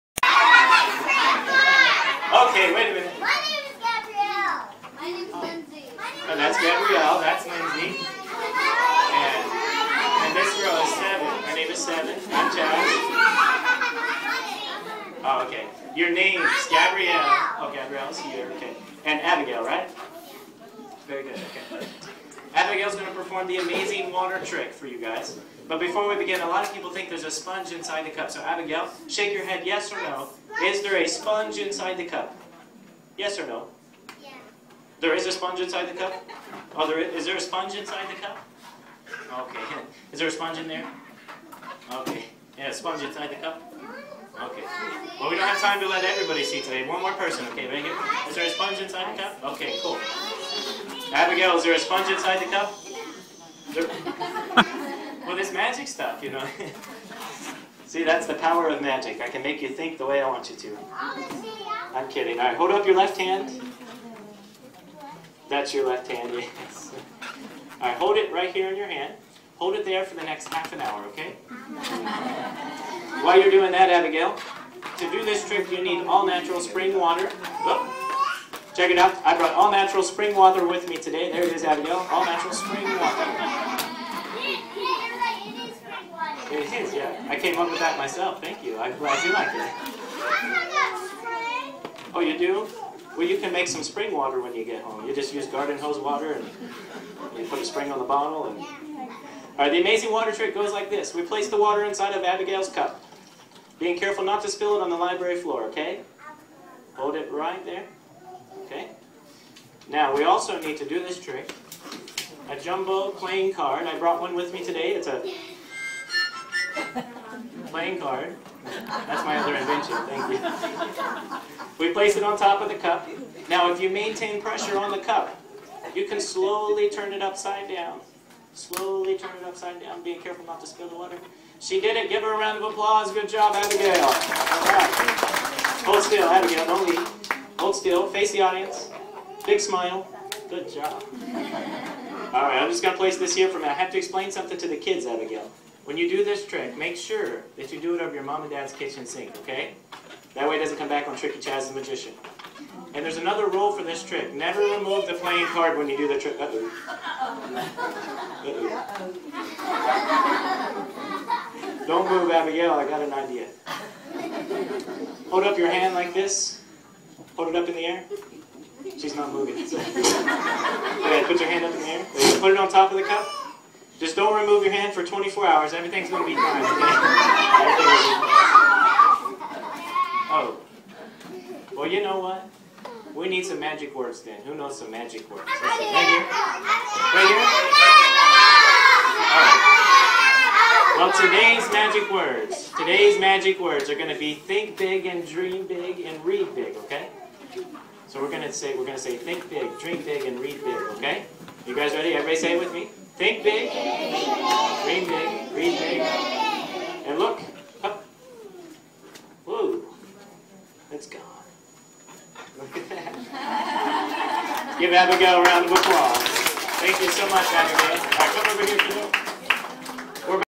Tricky Chaz at Willimastown Public Library
Tricky Chaz entertaining the kids with his fun & interactive comedy magic and juggling show.  Chaz can be seen performing for children at libraries, schools, birthday parties, fairs and much more.